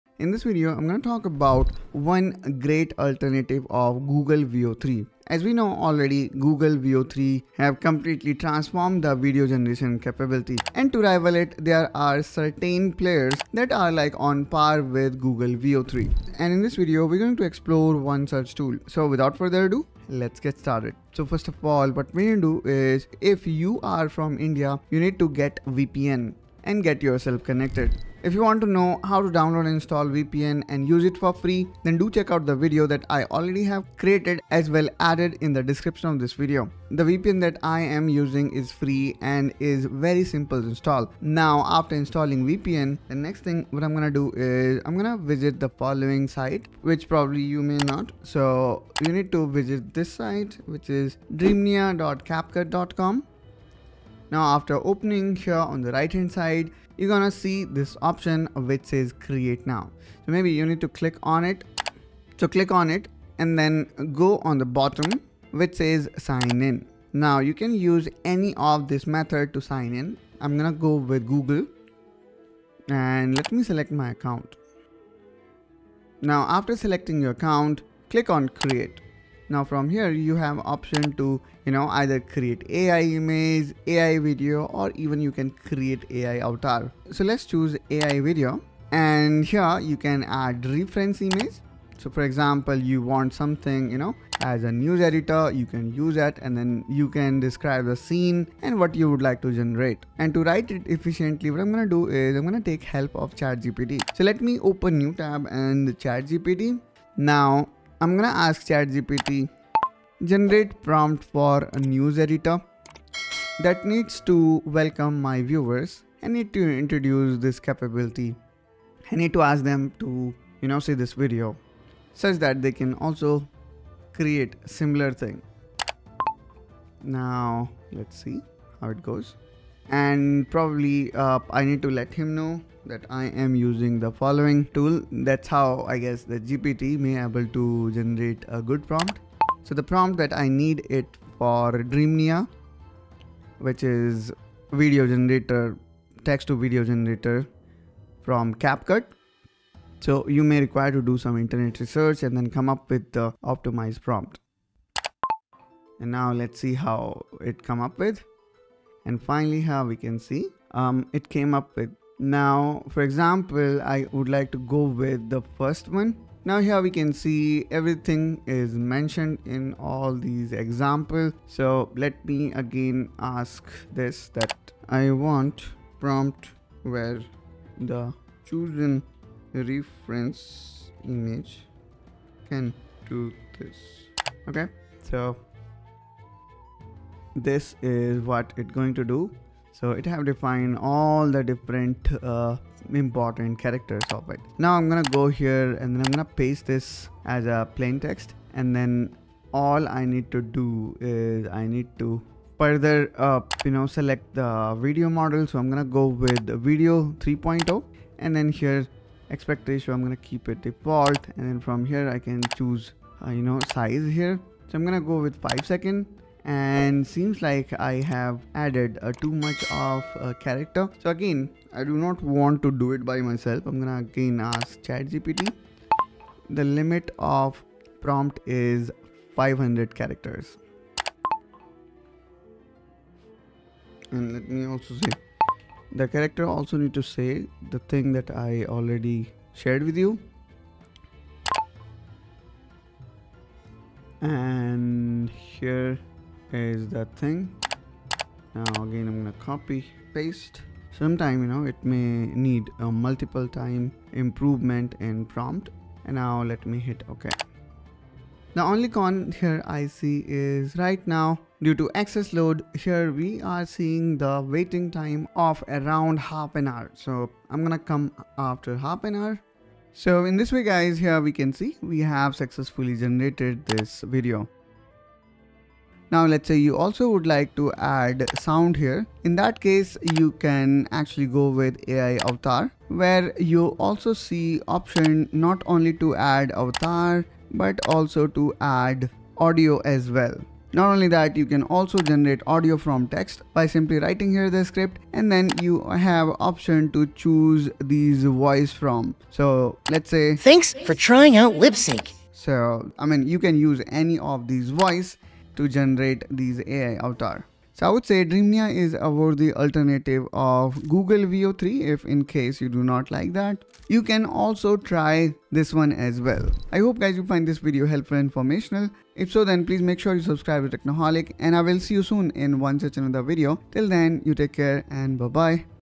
Dreamia: The BEST Free Alternative to Google VEO 3 AI Video Generator
Looking for a free and powerful alternative to Google V3 AI Video Generator?  In this video, I will walk you through Dreamia by CapCut, an amazing tool that lets you create AI-generated videos, avatars, and even voiceovers.
You will learn:
✅ How to access Dreamia in India using VPN
✅ How to generate AI videos with prompts
✅ How to optimize prompts with ChatGPT
✅ How to add AI avatars and voices for more engaging results

📌 Chapters
0:00 – Intro: Google Veo 3 alternative
0:20 – Why use VPN (for India users)
0:42 – Visiting DreamMia.caa.com
0:54 – Signing in & creating an account
1:19 – Choosing AI video creation
1:28 – Adding reference image & scene description
1:40 – Using ChatGPT for optimized prompts
3:36 – Adapting prompt for 500-character limit
4:18 – Wait time for generation
4:29 – Viewing generated video
4:35 – Adding sound & AI avatars
4:50 – Generating voice from text
5:07 – Final thoughts & recommendation
5:16 – Outro